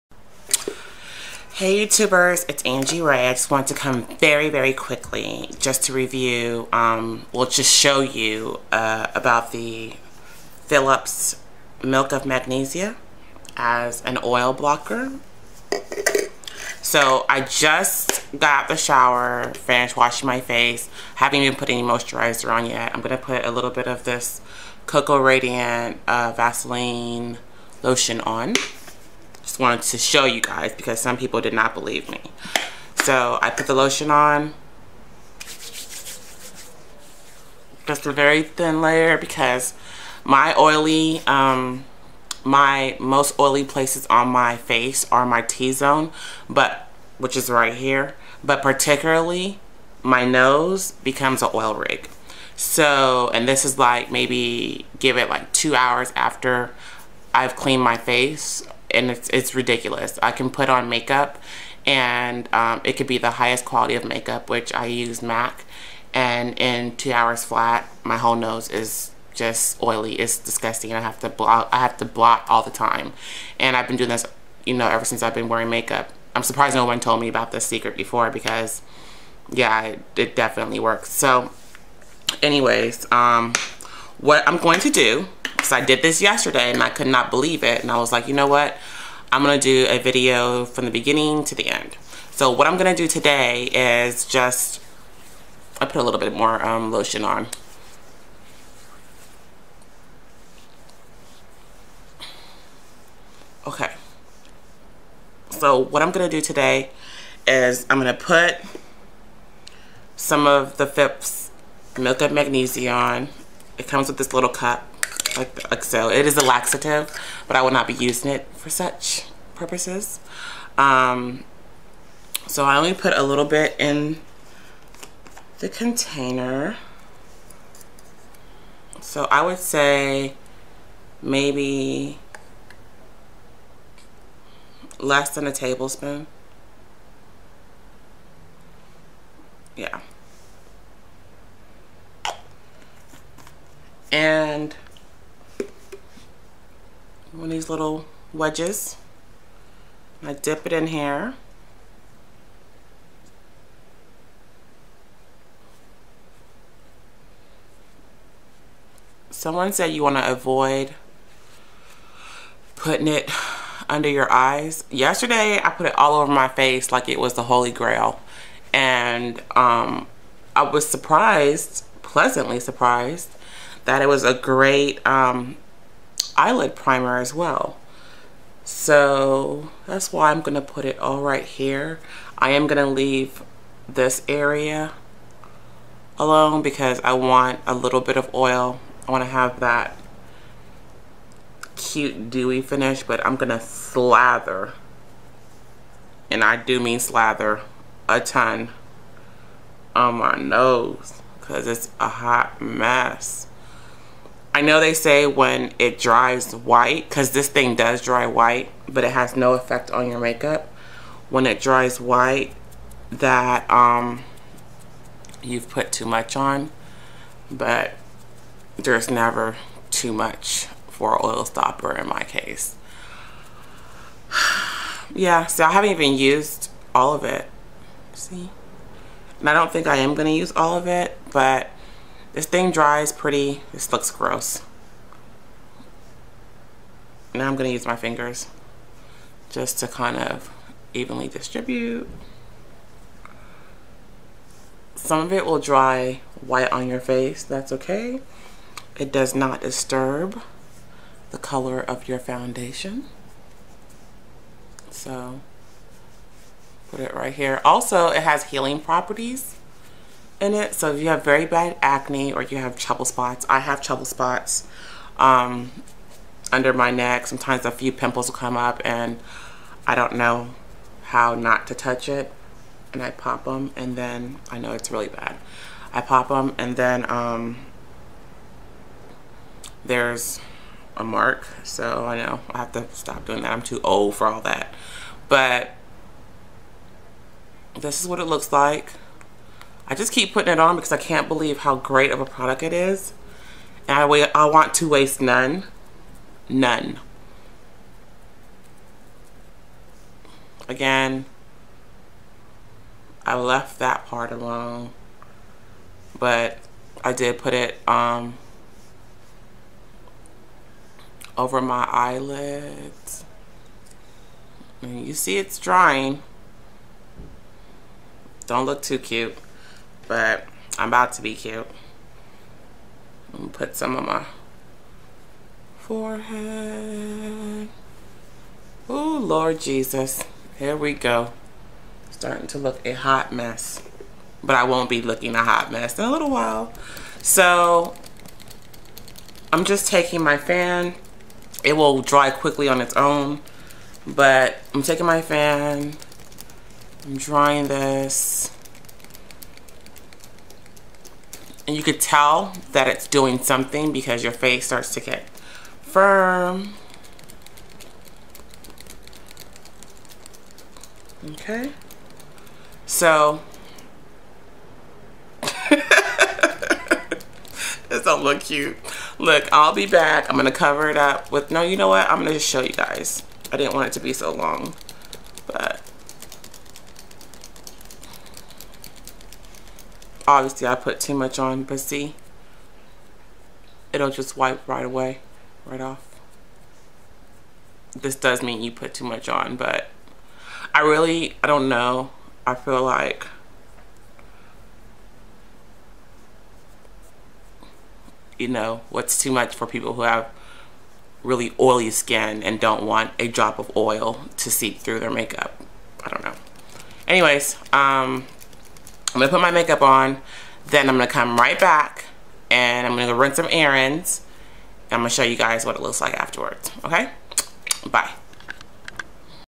An Anji Ray Review: Pt.1-- Rebuke The Oil Demons w/ Milk of Magnesia ! Yes Gawd!!
Part 1 of 3 videos on Anji's experience with Milk of Magnesia as a oil stopper.  I swear by this!  It works!  No more oily T-zone, No more makeup oxidizing!  Stay super matte all day long!  Thank you for taking the time to watch!

Product:

Phillips Milk of Magnesia
TOTAL PRICE--$6